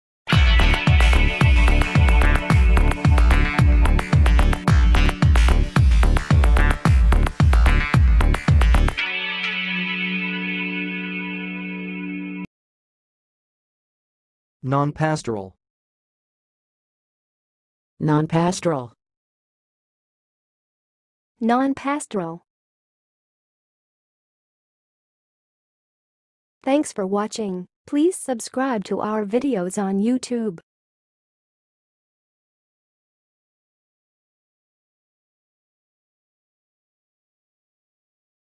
How to say "nonpastoral"! (High Quality Voices)
Watch in this video how to say and pronounce "nonpastoral"!